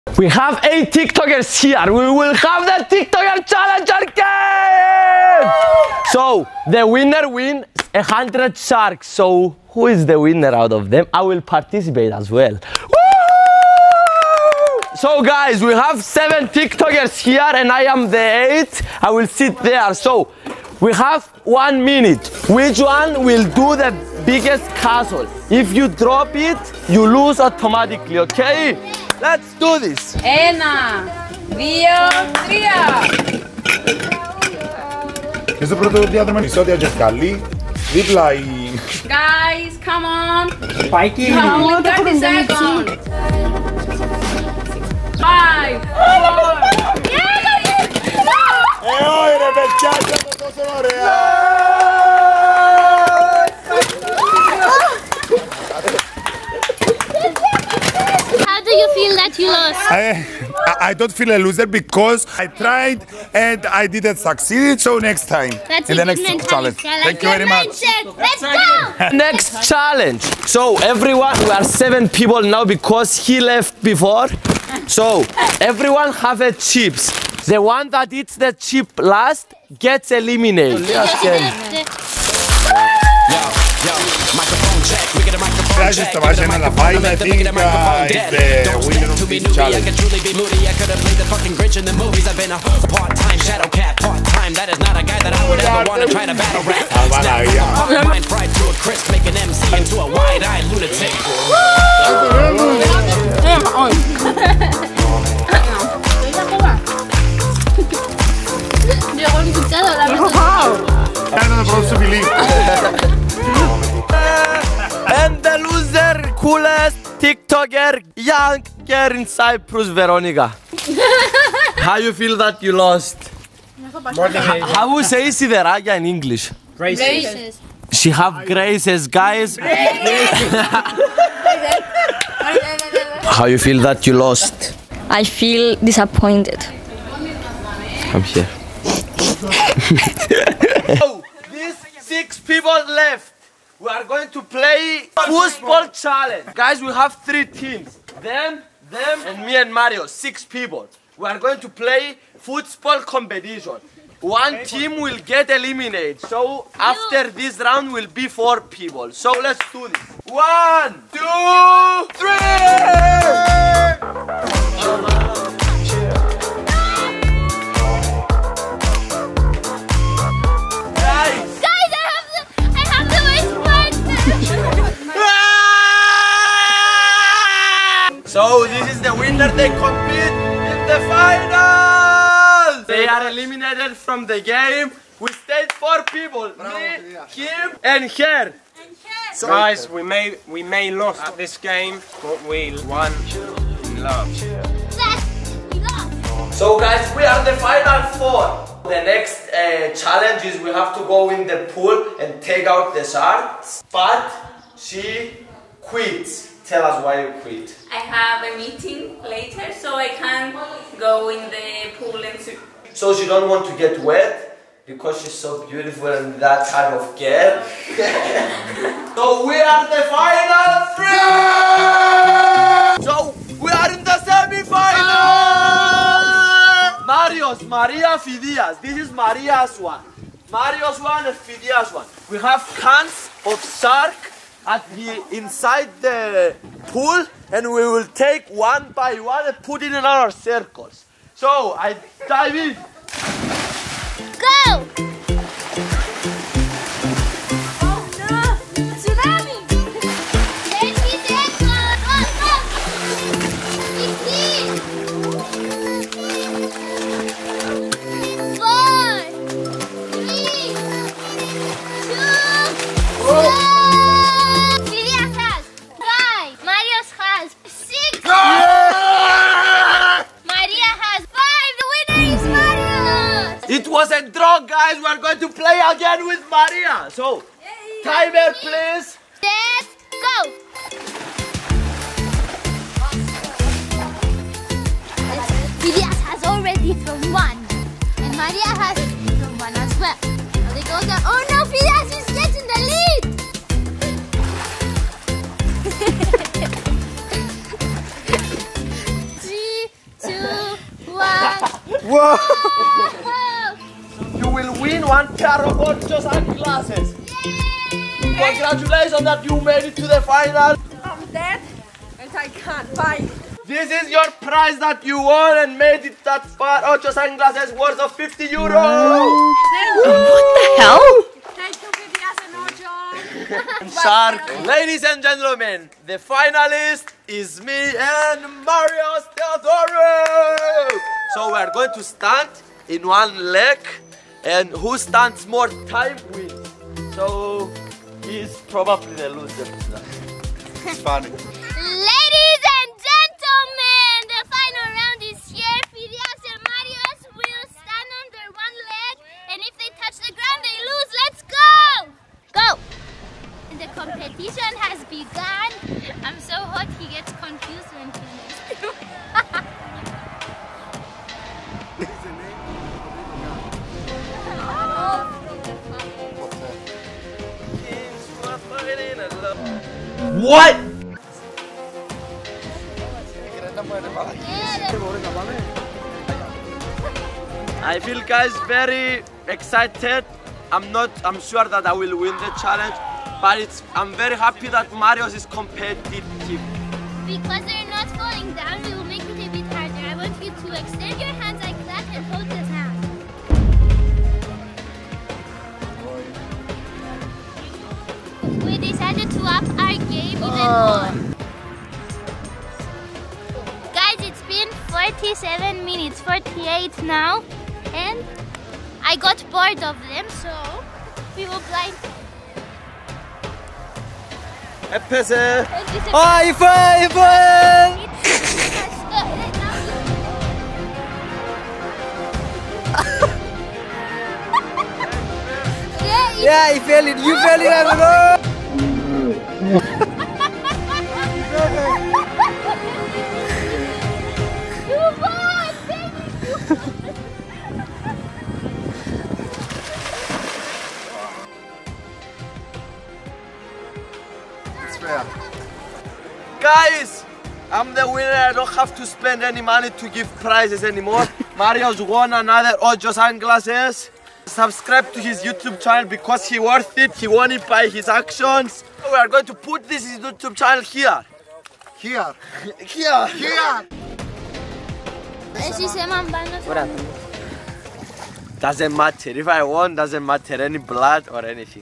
Last Tiktoker To Fall Wins €100!
8 Tiktoker and the Finalists get €100 Worth of Shark

Edited by: Laxminath dangol